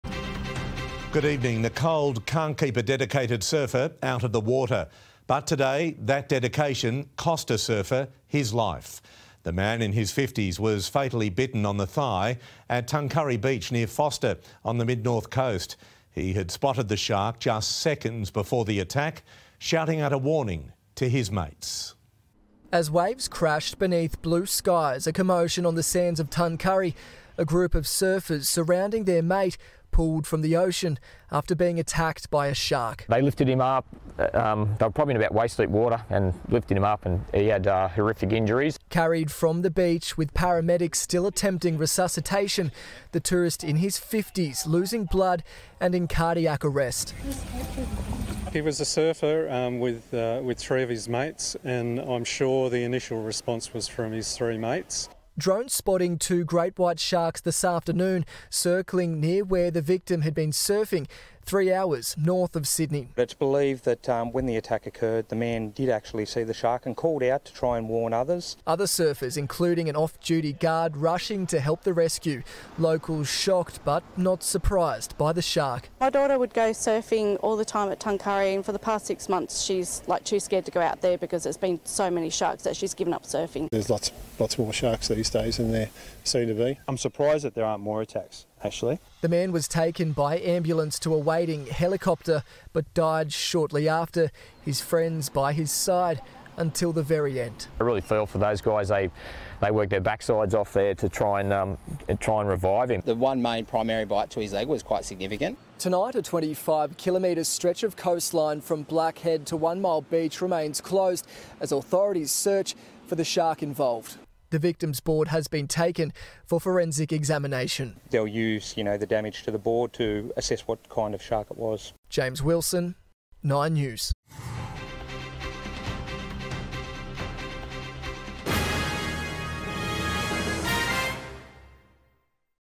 Four-metre great white spotted after man killed in shark attack | 9 News Australia
A man has died after he was attacked by a shark on the NSW Mid North Coast.